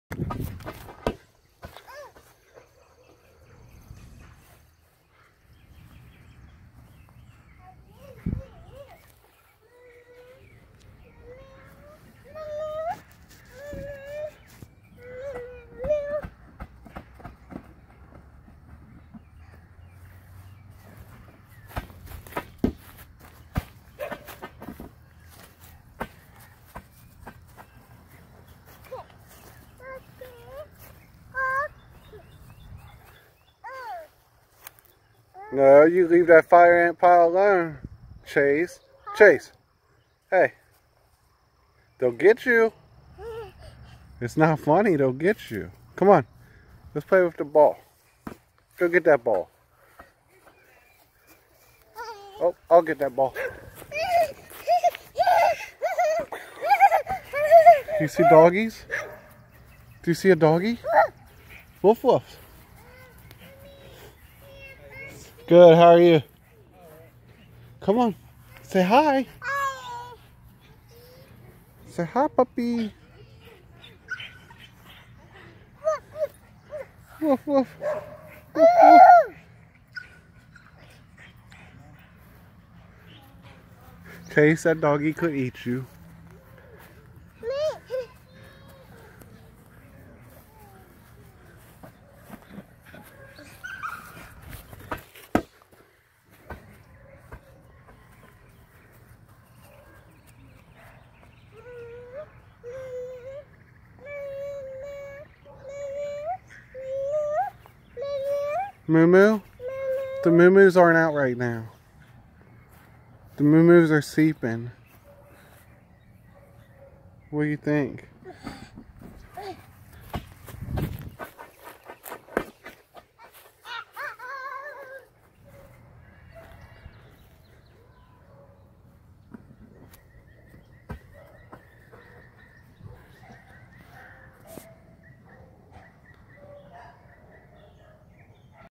Galaxy S8 1080p Video Test 60fps
Just playing around with my son in the front yard. Shooting video with the Galaxy S8 rear facing camera.  Something a lot of  people would use the camera for Full review coming soon.